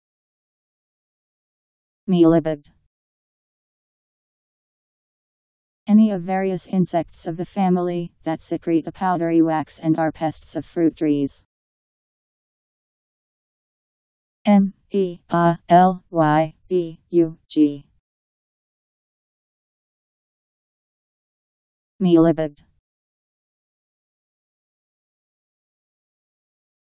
Mealybug Meaning
Video shows what mealybug means. Any of various insects of the family , that secrete a powdery wax and are pests of fruit trees..  Mealybug Meaning. How to pronounce, definition audio dictionary. How to say mealybug. Powered by MaryTTS, Wiktionary